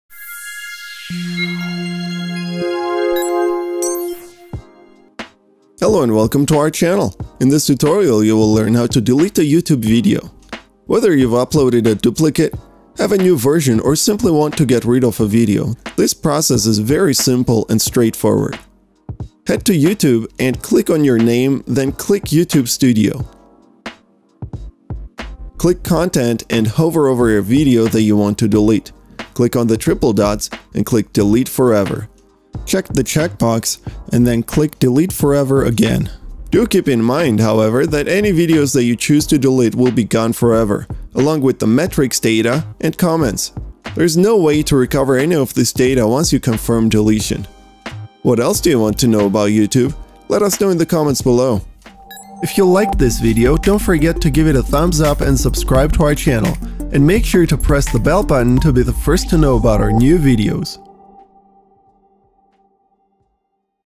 How to Remove Videos from YouTube Channel | How to Delete a YouTube Video on a Computer
In this tutorial, you will learn how to Delete a YouTube Video.

Whether you’ve uploaded a duplicate, have a new revision or simply want to get rid of a video, the process is very simple and straightforward.

Head to youtube and click on your name then click Youtube Studio. Click Content and hover over a video that you want to delete. Click on the triple dots and click Delete Forever. Confirm if asked.

It’s that simple! Do keep in mind, however, that any videos that you choose to delete, will be gone forever, along with the metrics data and comments! There’s no way to recover any of this data once you confirm deletion.

❓💬 What else do you want to know about YouTube?